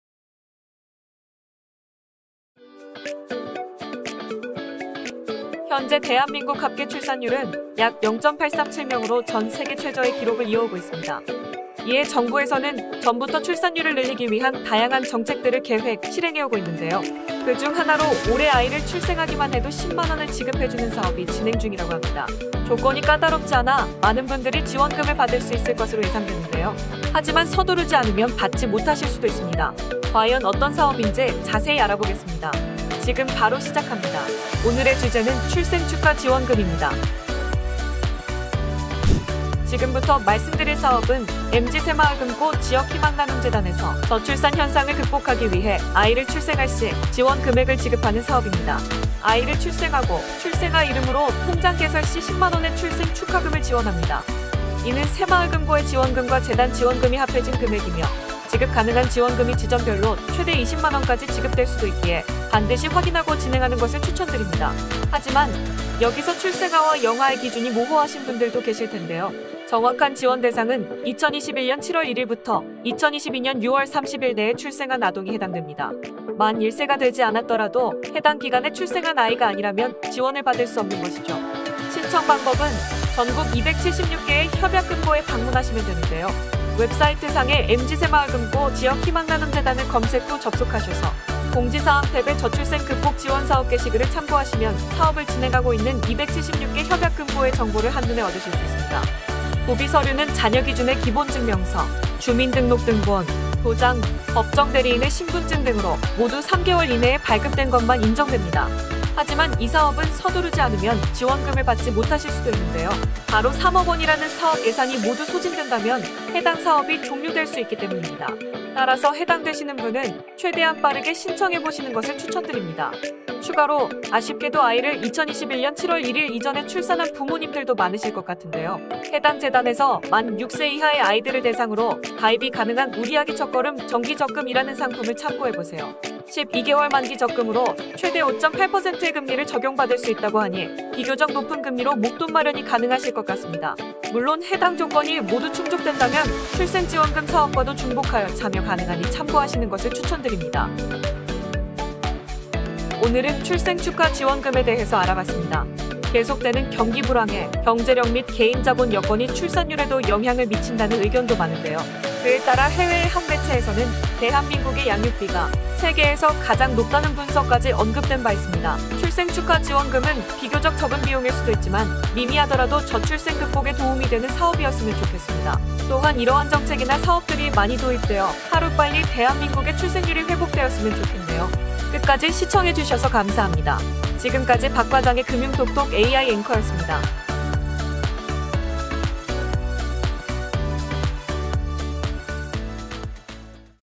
임산부 출산 혜택 똑똑히 받아가세요!! 모르면 손해 👶🏻
대한민국의 가장 큰 숙제!!
👉🏻  바로 저출생 문제를 극복하기 위해
"출생축하 생애 첫 통장발급 지원 사업"을 진행한다고 합니다!
자세한 내용은 영상 끝까지 확인해보시고 신청해보세요!
좋아요 구독 댓글은 저희에게 큰 힘이 됩니다! 🙏

00:00 - 인트로
00:39 - 출생축하 지원금
01:07 - 지원 대상
01:26 - 신청 방법
01:40 - 구비 서류
01:50 - 신청기한
02:08 - 우리아기 첫걸음 정기적금
02:32 - 아웃트로